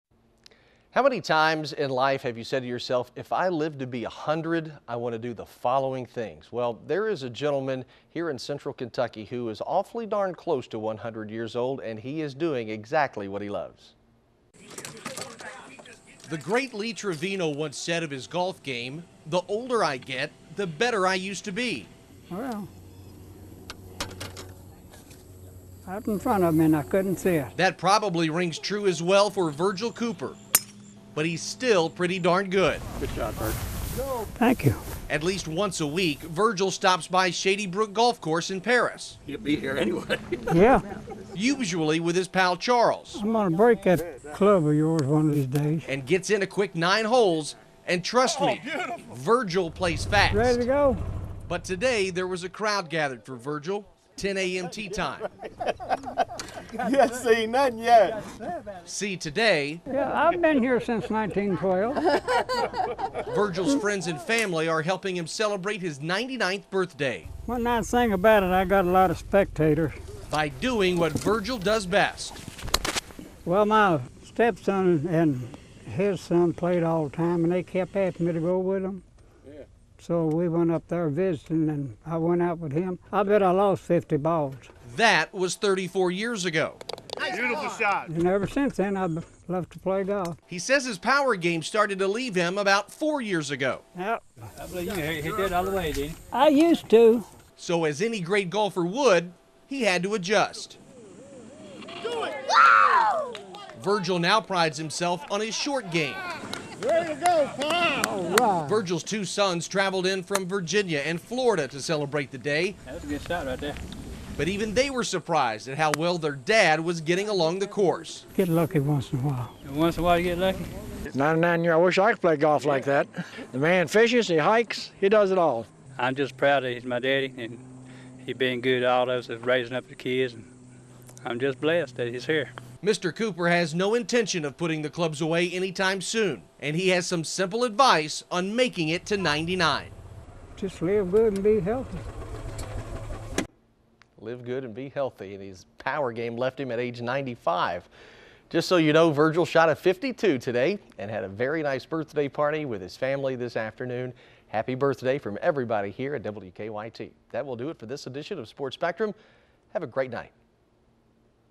Virgil Cooper
Paris Kentucky Resident Virgil Cooper is celebrating his 99th birthday doing what he loves to do most...

wkyt Reporter Brian Milam; Photographer John C. Wilson